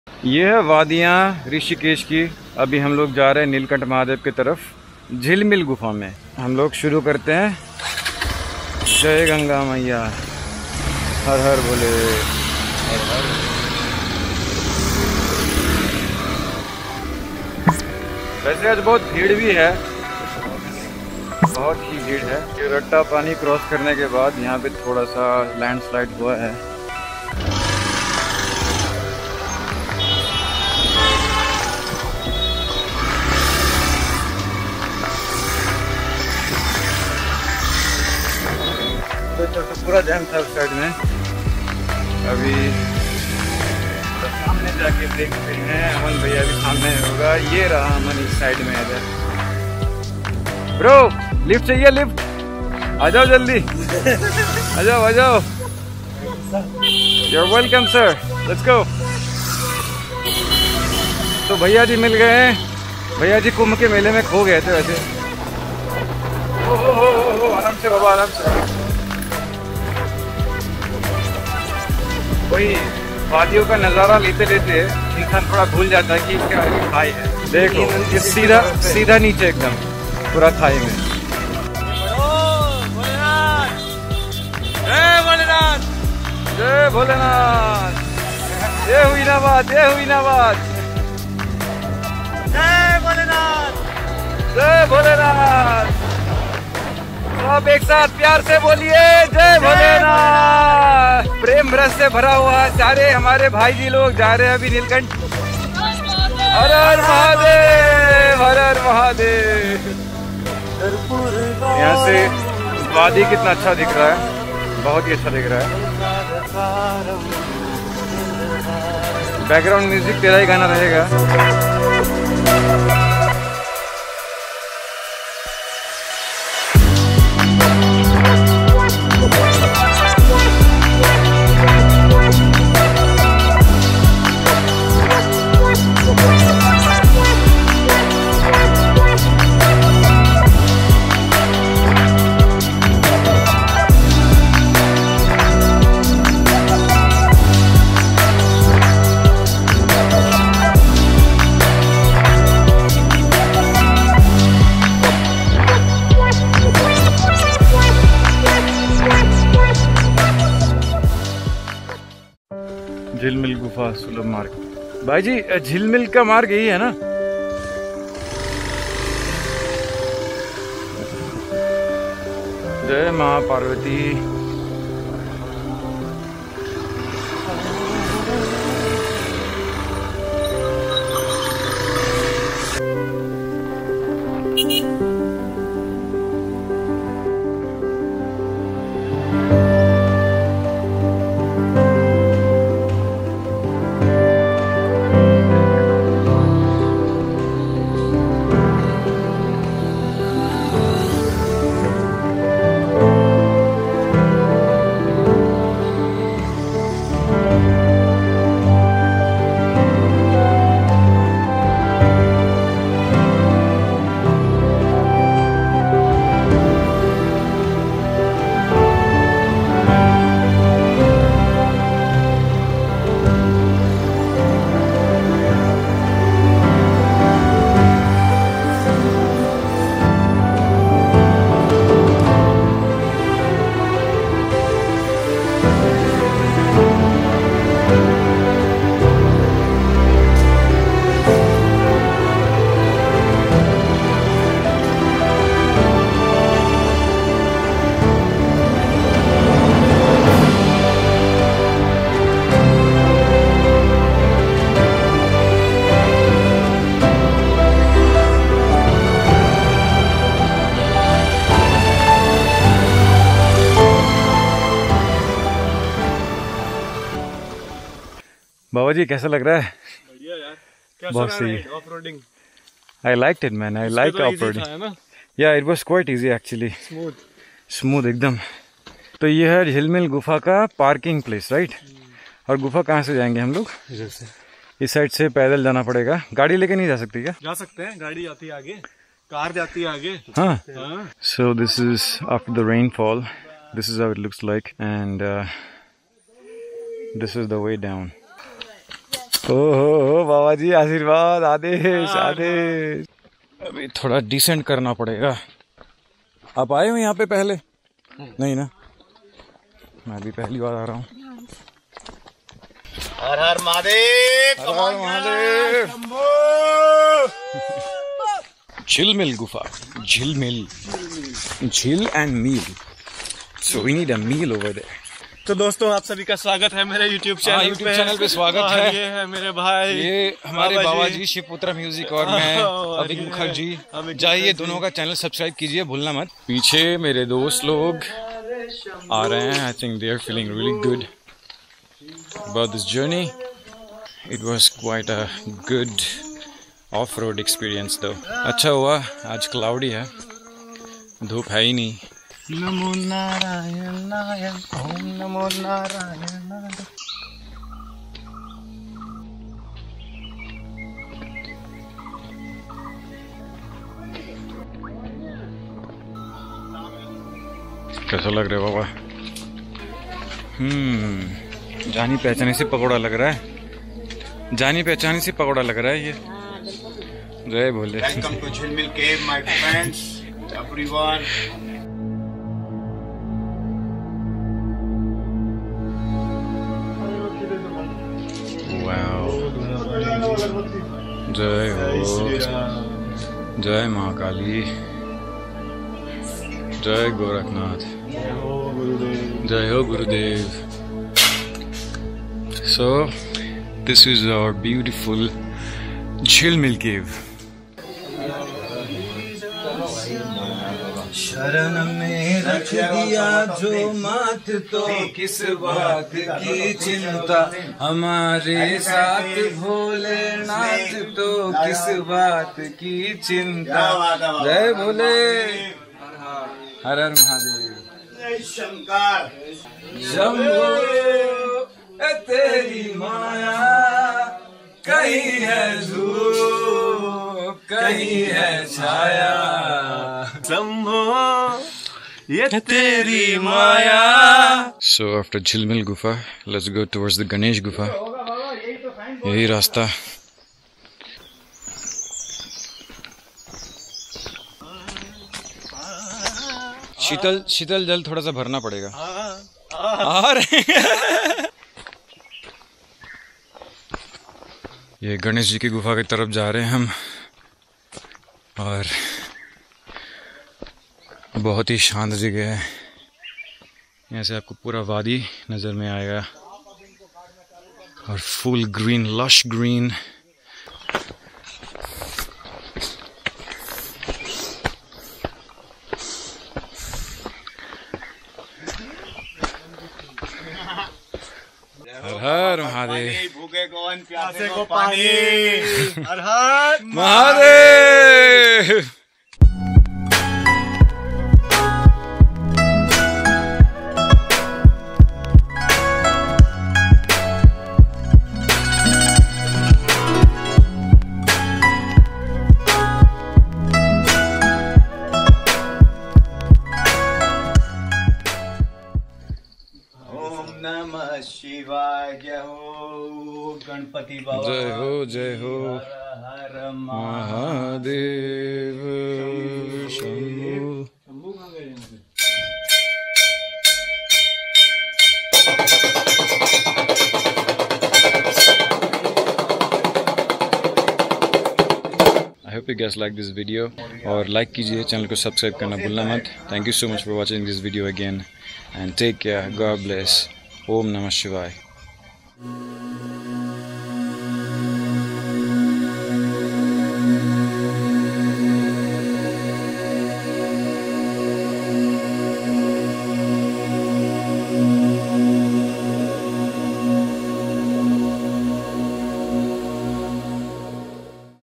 Jhilmil Cave | Rishikesh | Avik Mukherjee
Jhilmil Gufa (cave) is purely a gift of nature. This amazing place is very divine because of its spiritual significance and also for nature lovers and vloggers.

Guru Gorakhnath had performed severe austerities to reach the pinnacle of Yog Sadhana which is a blessing to mankind.

About 25km from Tapovan (Rishikesh) this place can be reached via Neelkanth road and approximately 4km from Neelkanth Mahadev mandir. A must visit place if you're in Rishikesh.